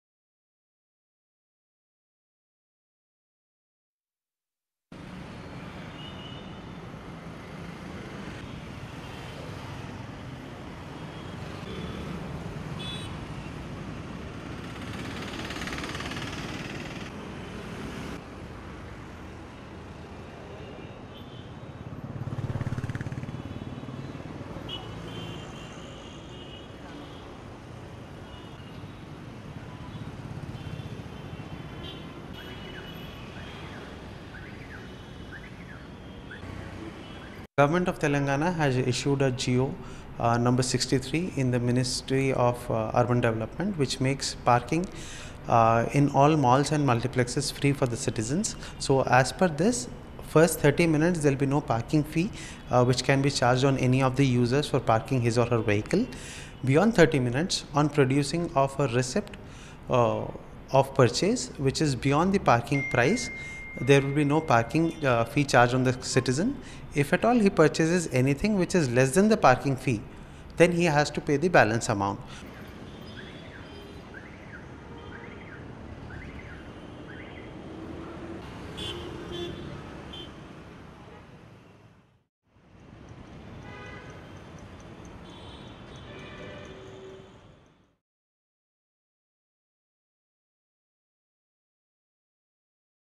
GHMC successfully implements free parking policy at malls - Telangana News
Hyderabad (Telangana), Apr 20 (ANI): Free parking policy of the Telangana government is being successfully implemented by the Greater Hyderabad Municipal Corporation (GHMC).